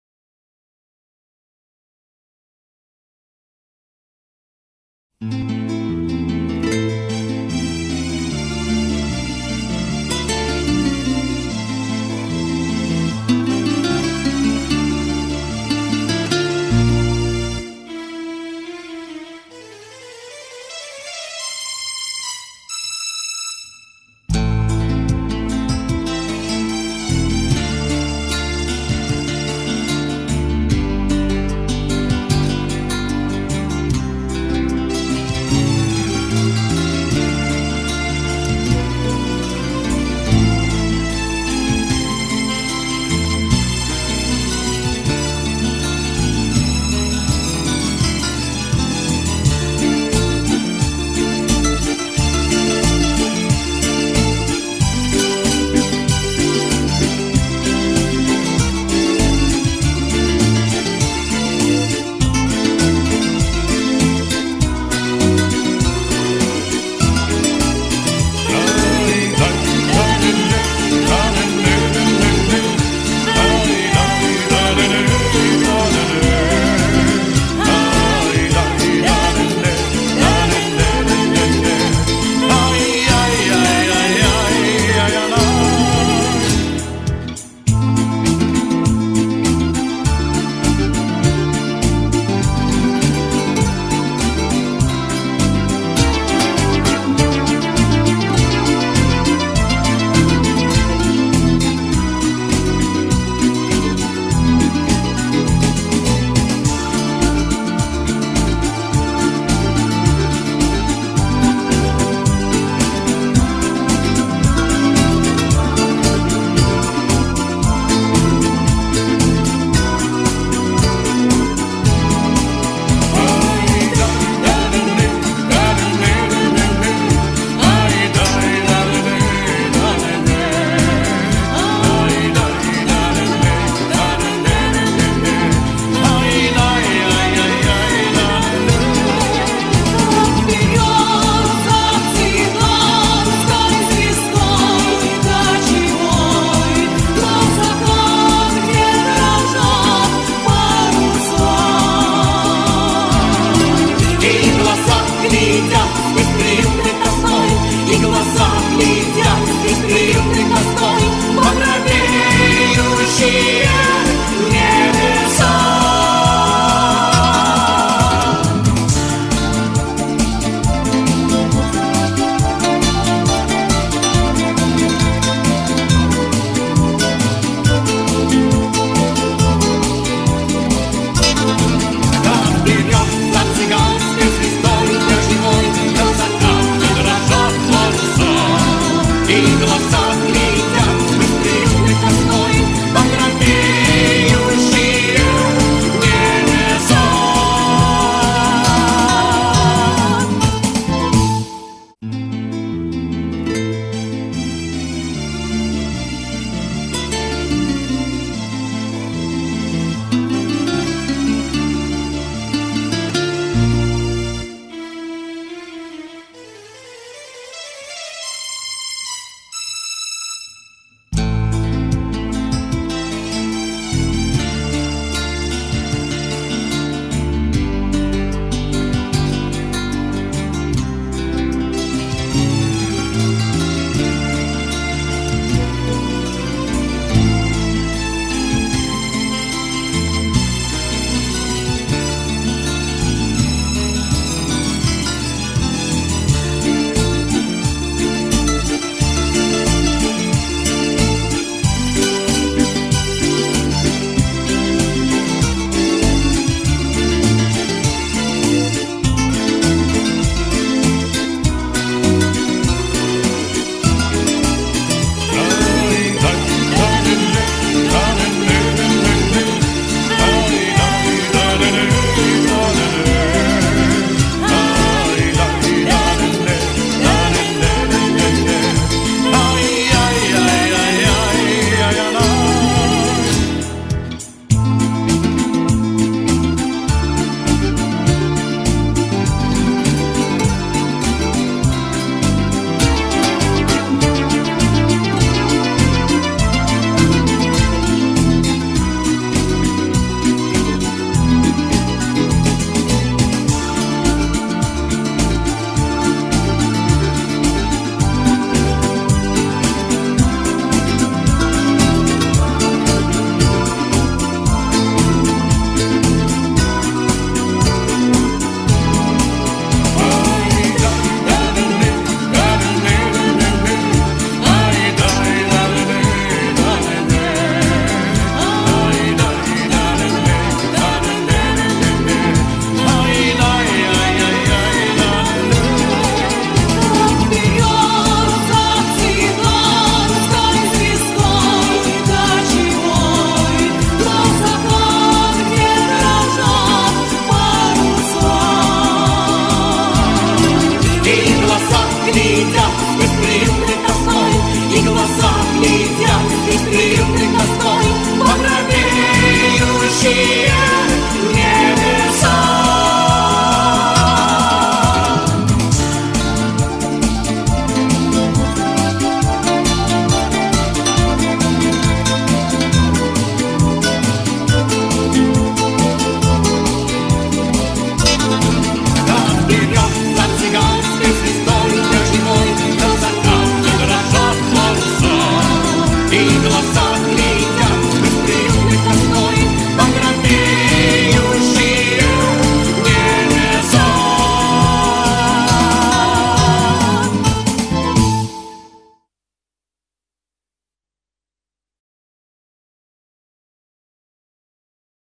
Квиллинг. Мохнатый шмель. Мастер-класс.
Как сделать мохнатого шмеля в технике квиллинг. Мастер-класс.